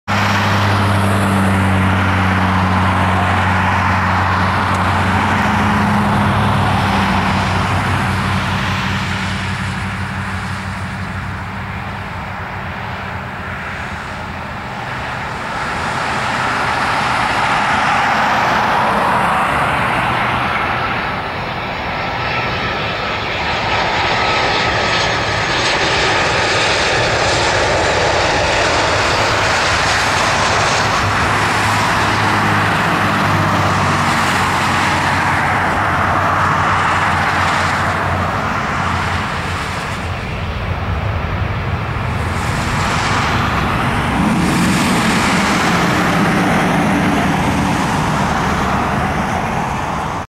Southwest Airlines (New Mexico One) 737-700 [N781WN] Landing Portland Airport (PDX)
Flight: SWA5466 SJC-PDX
Landing on runway 10L during the south runway closure.